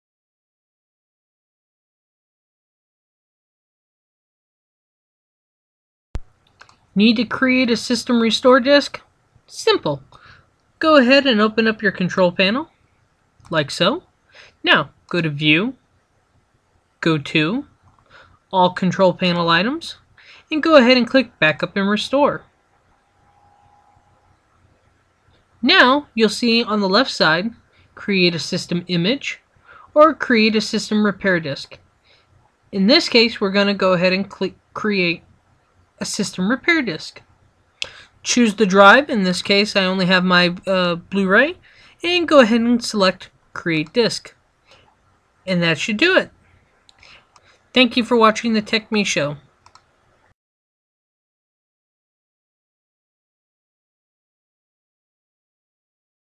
Create a System Restore Disc easily in Windows 7
Today we teach you quickly how to make a Windows 7 System Restore Disc in case you may encounter system issues. Never hurts to be prepared!